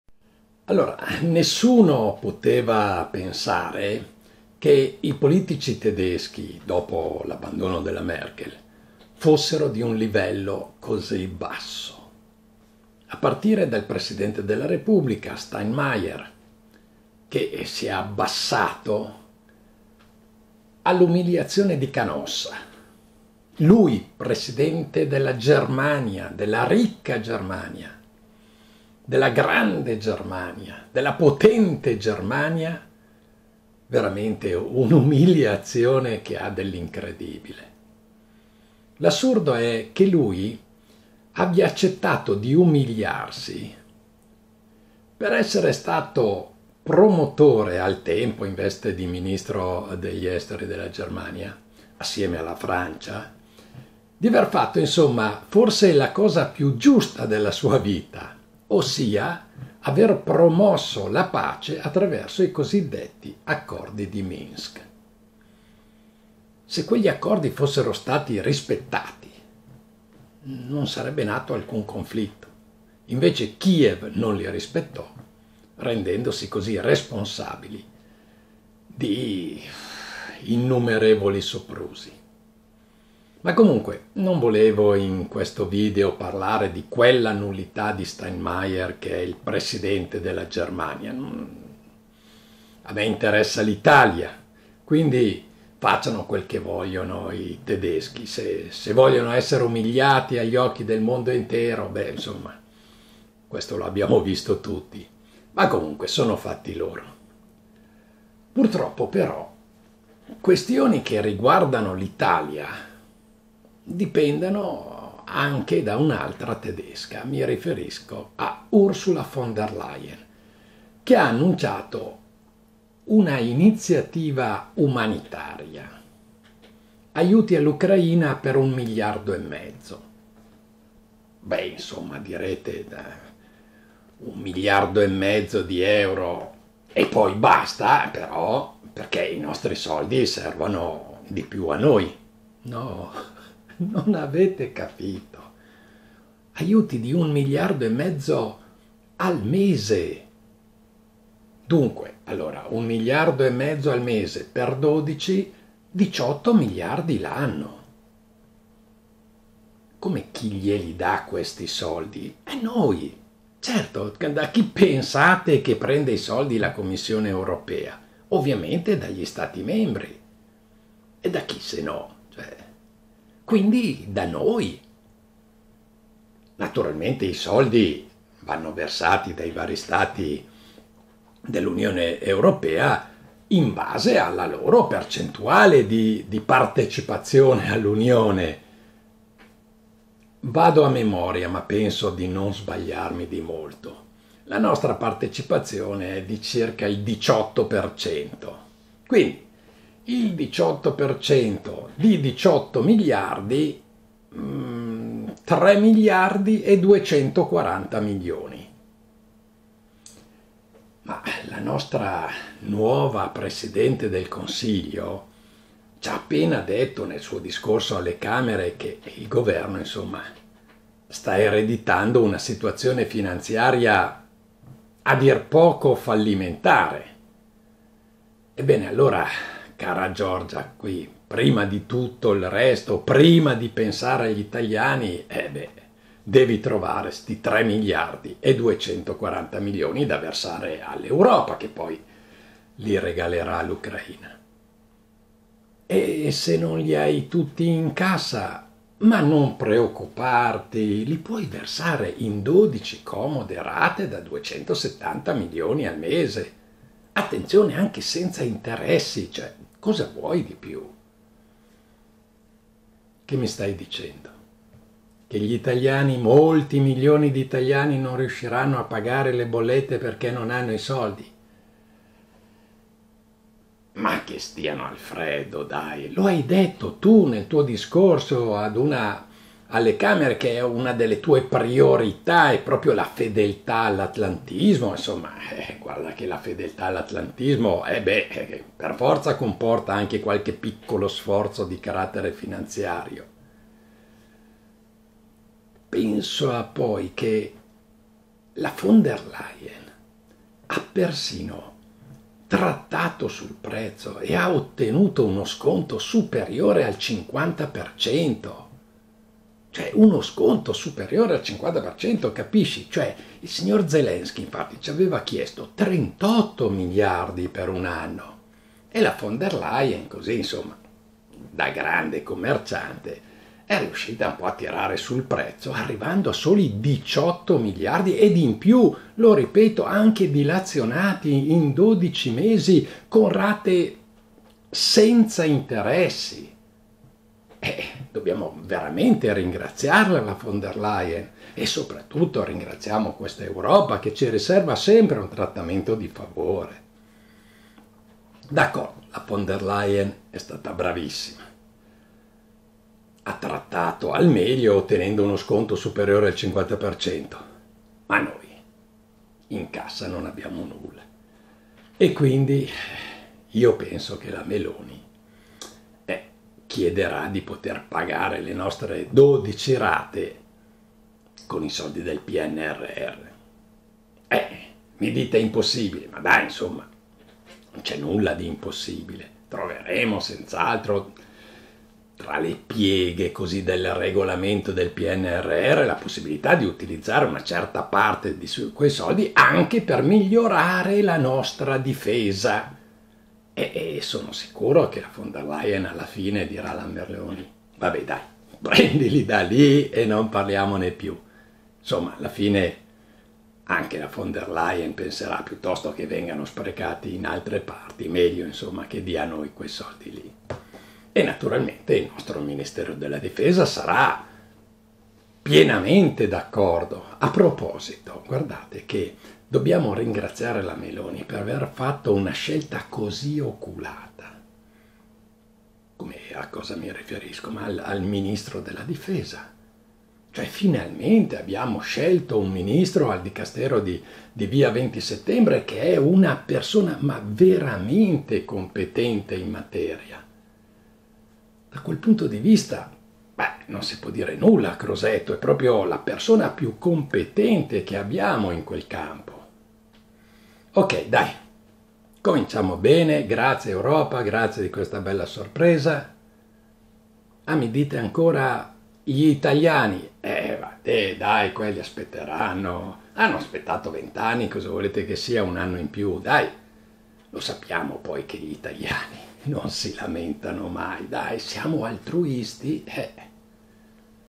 L’EUROPA BATTE SUBITO CASSA
Mentre il Presidente della Germania va a Canossa, la Presidentessa della Commissione europea, anch’essa tedesca batte cassa.
E’ riuscita ad ottenere uno sconto superiore al 50%, di più non le si poteva chiedere, anzi, ha anche ottenuto che i 18 miliardi possano essere versati in dodici comode rate mensili … e senza interessi!!!
Purtroppo la Meloni non ha ereditato nulla, ma fra le pieghe del PNRR … vedrete …  che salterà fuori la nostra parte.